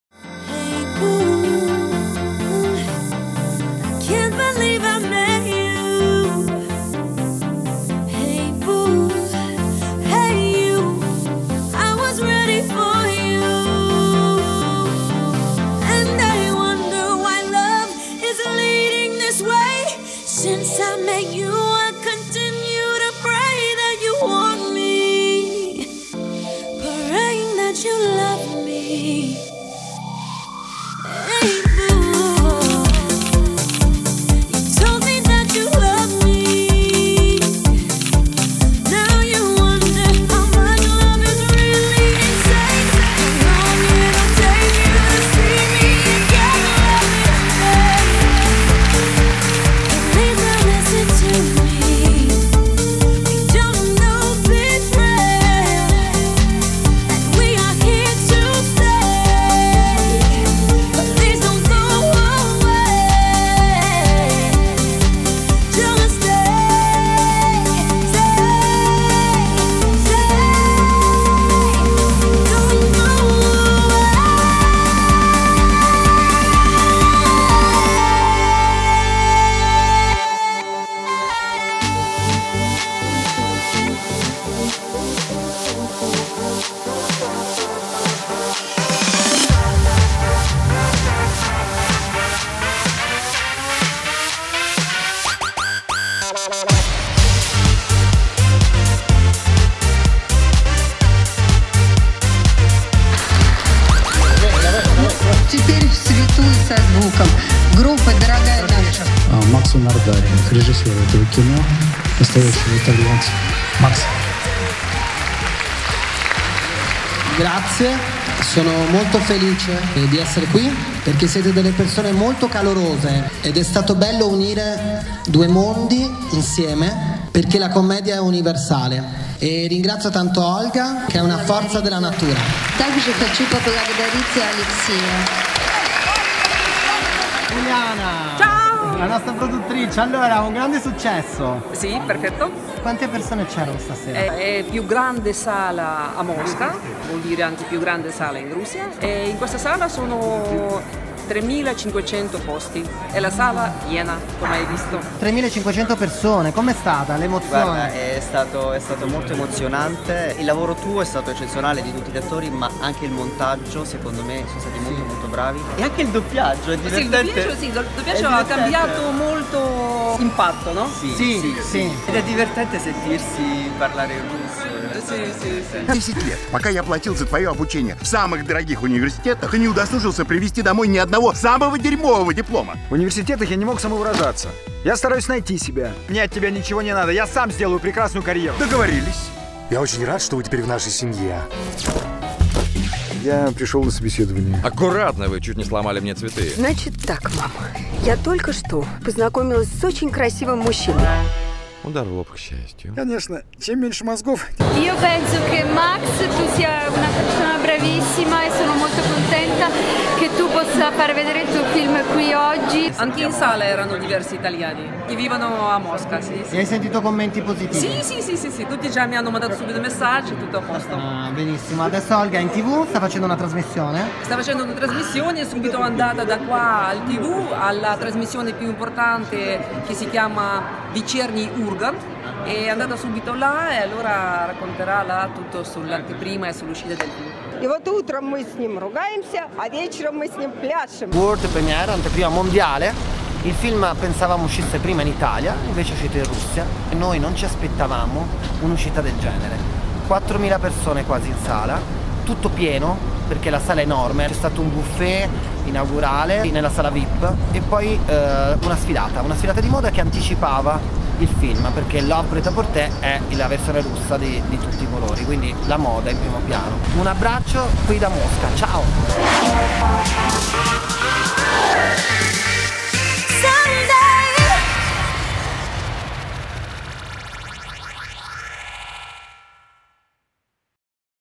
Anteprima mondiale film LIUBOV PRET A PORTE' di Max Nardari-Mosca (Russia)
Anteprima eccezionale in Russia a Mosca con 4000 persone. Il film Liubov Pret a Porte esce in Russia in 470 sale a marzo 2017 , distribuito da Paradise. Film per la regia di Max Nardari. Fotografia di Giancarlo Ferrando. Sceneggiatura di Max Nardari, Alba Calicchio e Daniele Malavolta.
Sinossi:
Giorgio è bel ragazzo bugiardo, talmente bugiardo che arriva persino a fingersi gay e diventare “Giorgy” per trovare un lavoro all’interno di una casa di moda!
Olga invece è una bella ragazza russa, arrivata in Italia per lavorare in quella stessa maison come vice presidente. Giorgio, vittima delle sue stesse bugie, per conquistare il cuore di Olga, sarà costretto a diventare per la prima volta una persona sincera.